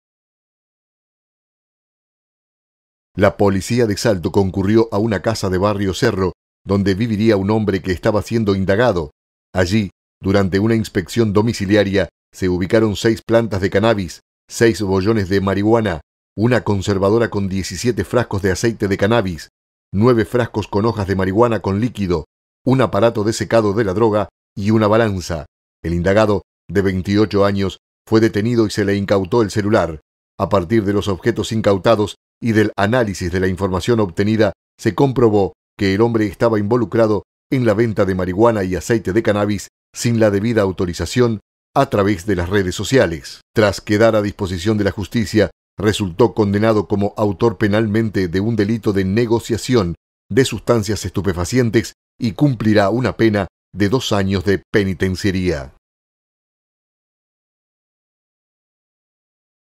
Hombre procesando por vender marihuana y aceite de cannabis sin autorización
Mediante un procedimiento policial se detuvo a un joven que venía siendo investigado por la venta de marihuana y cannabis de forma ilegal. Culminadas las actuaciones, se lo condenó a dos años de penitenciaría.